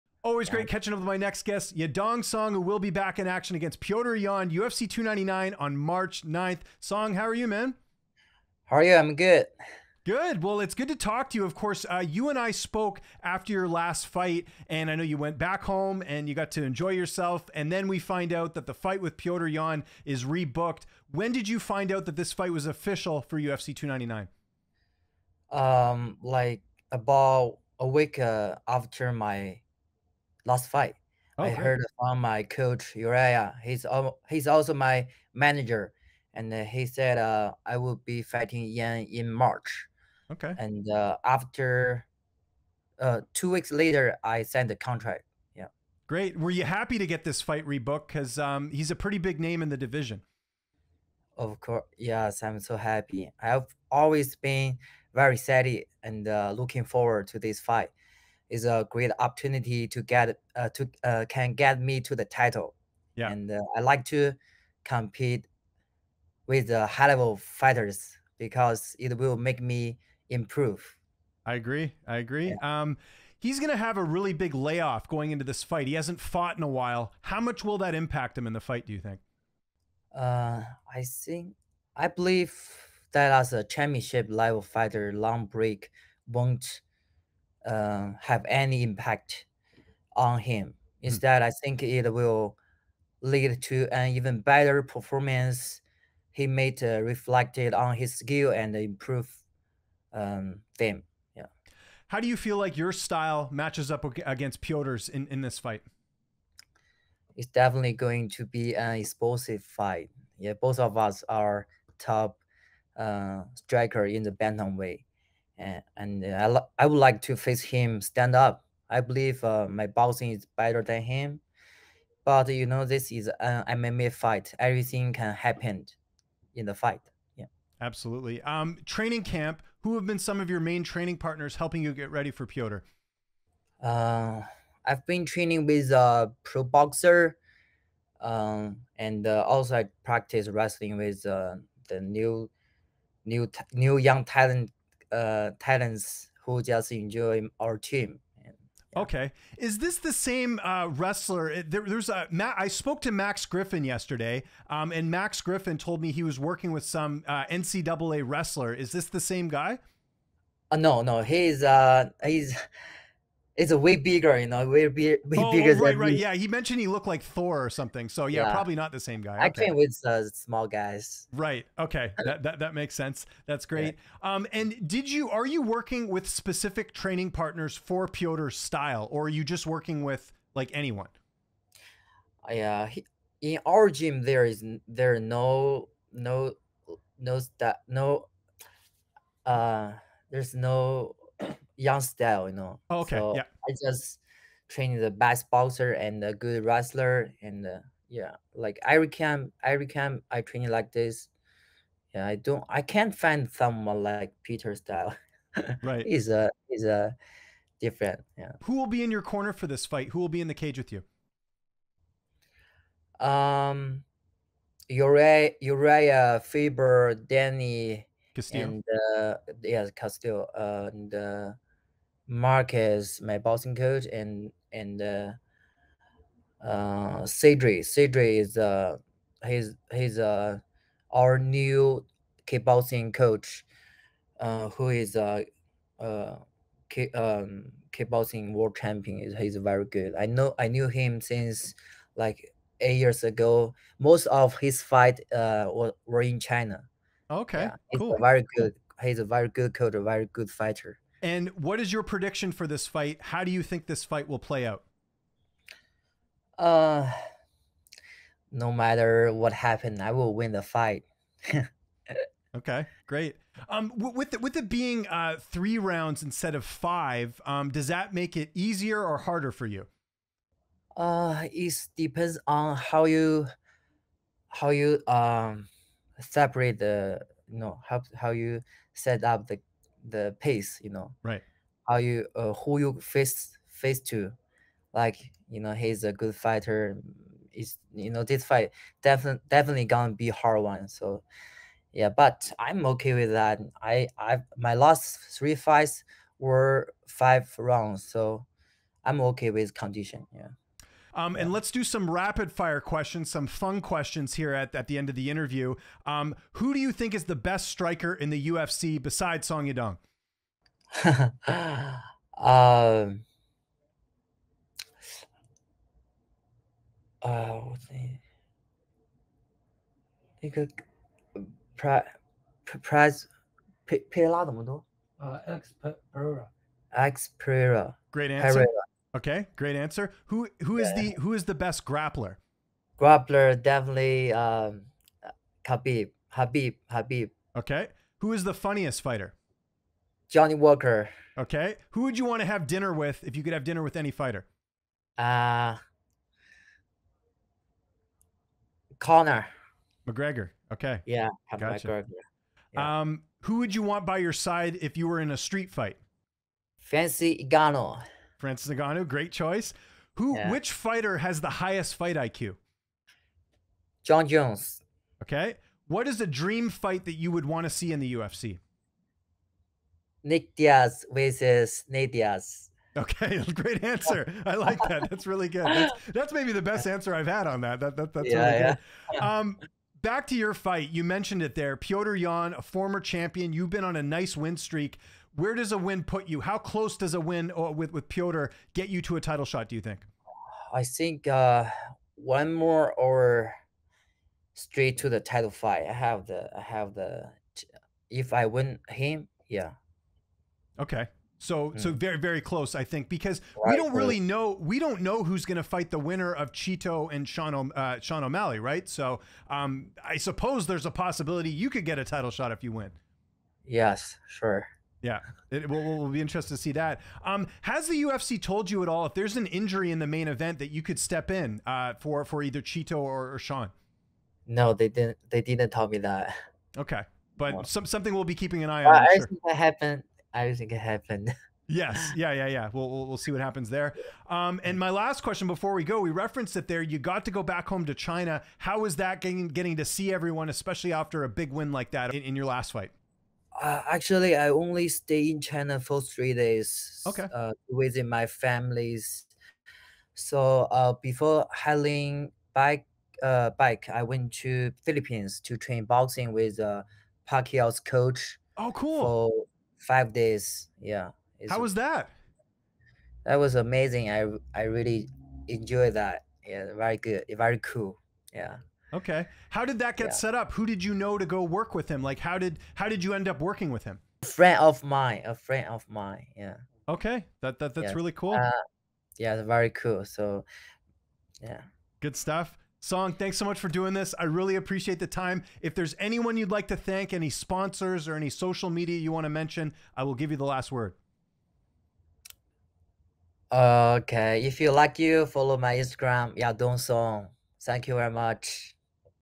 Yadong Song previews UFC 299 matchup vs. Petr Yan & talks title aspirations
Yadong Song (27-7-1) discusses his bantamweight fight against Petr Yan (16-5) at UFC 299 on Mar. 9. Timestamps below

0:00 - Intro
0:15 - When was the fight with Petr Yan offical?
1:16 - How much will the layoff impact him?
1:44 - Style matchup
2:10 - Training camp
3:29 - Corner
4:10 - Prediction
4:28 - Easier or harder that it's 3 rounds instead of 5?
5:01 - Rapid Fire Questions
6:22 - Where does a win over Yan put you?
6:57 - Have you been tabbed as a backup for O'Malley or Vera?
7:16 - How was going back home to China?
7:49 - Working with Manny Pacquio's boxing coach
8:02 - Sponsors & Socail Media